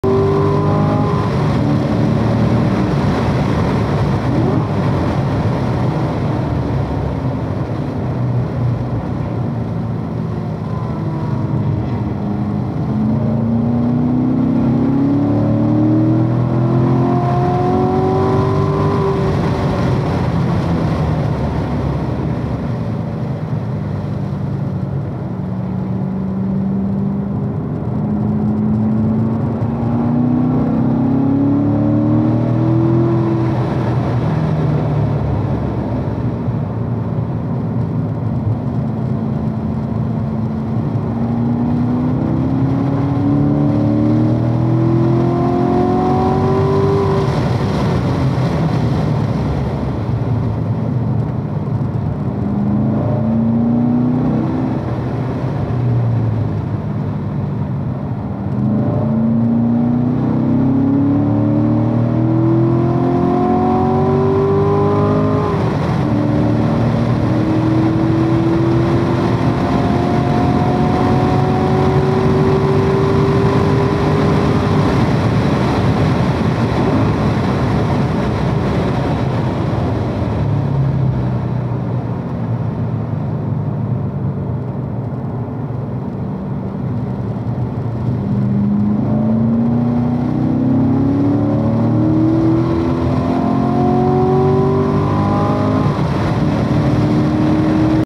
2015 C7 Corvette Stingray Z51 First time at Palm Beach International Raceway
2015 C7 Corvette Stingray Z51, M7 transmission
May 12, 2018
First time at Palm Beach International Raceway
Ignore the lap timer... I didn't set the finish line properly on the PDR.
Lap time looks to be about 1:34